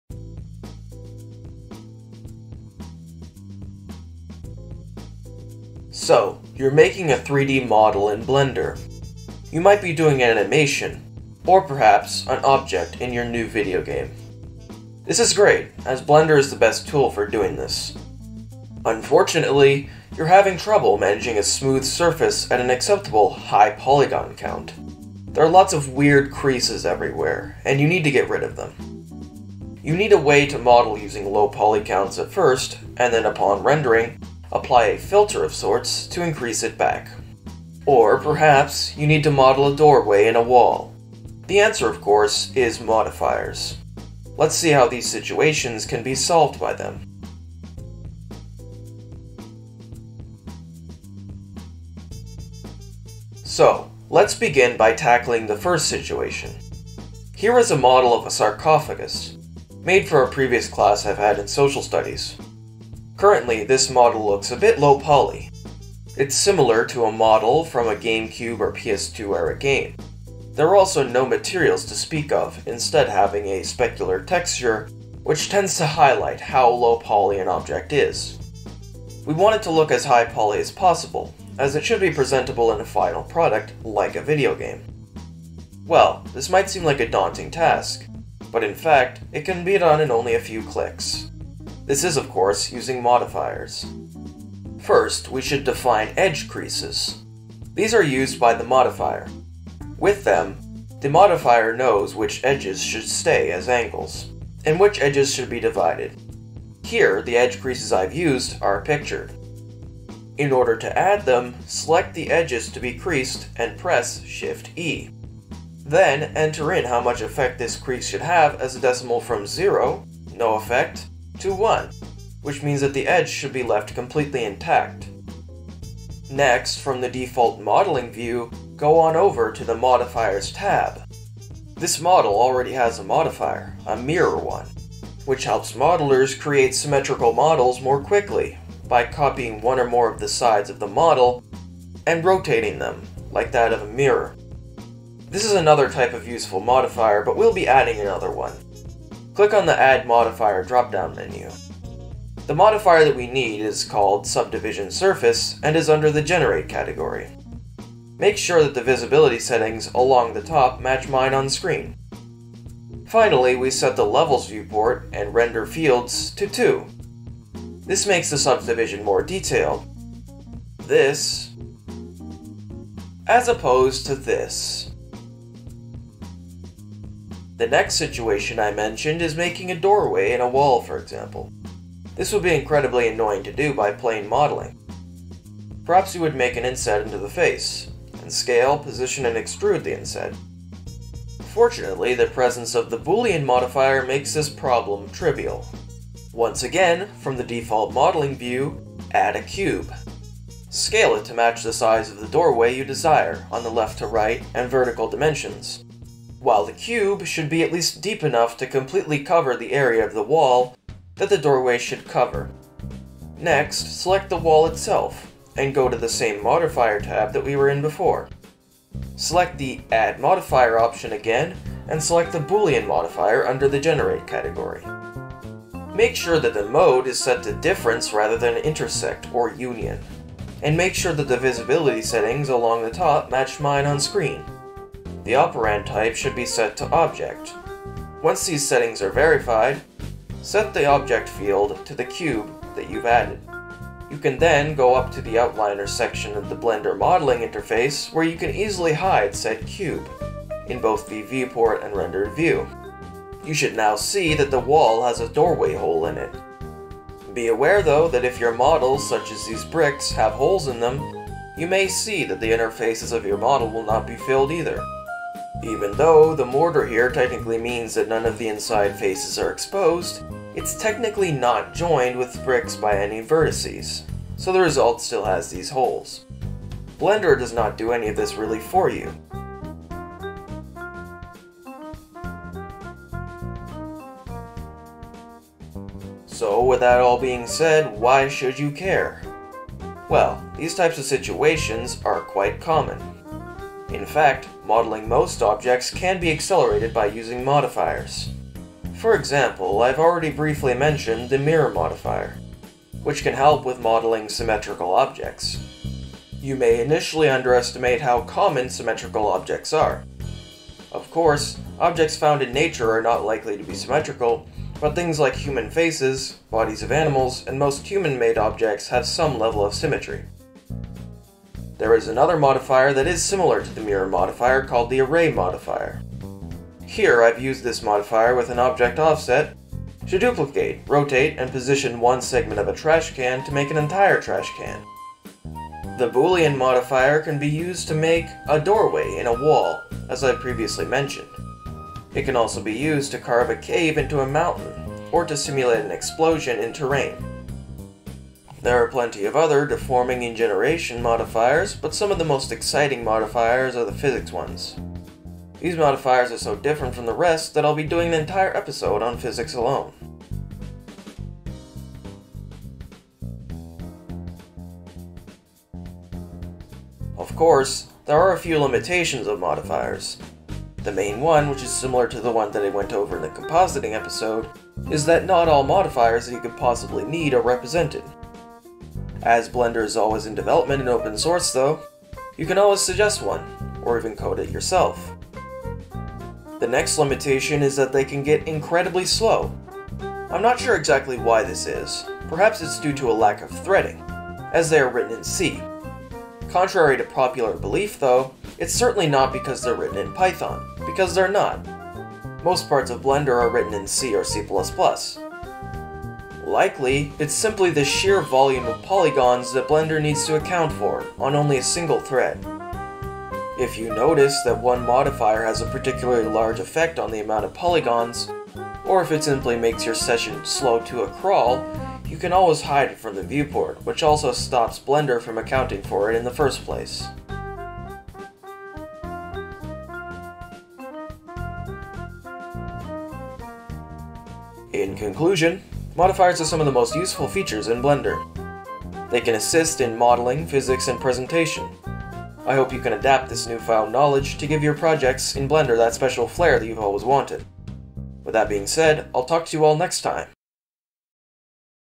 Blender - Modifiers - What are they, and Why are they Important?
If there are any issues with the credits featured for this video, please let me know. I will be happy to update them.